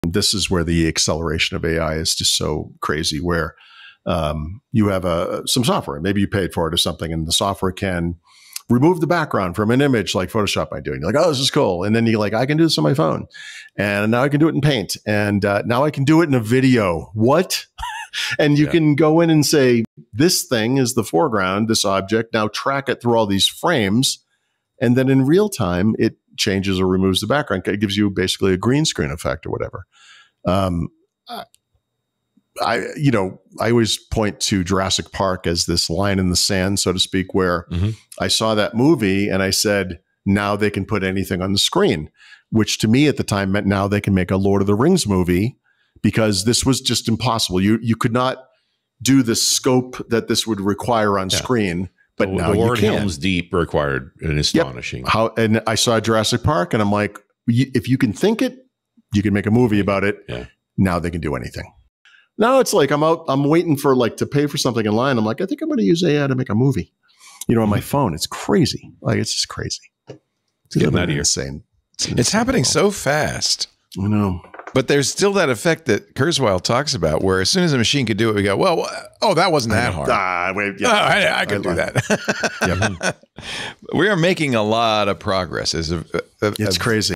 AI Accelerates Creativity Like CGI Did for Films
We explore how AI revolutionizes image and video editing, enabling filmmakers to create stunning effects easily. From background removal in real time to the power of imagination in cinema, discover how accessible technology is reshaping creative possibilities.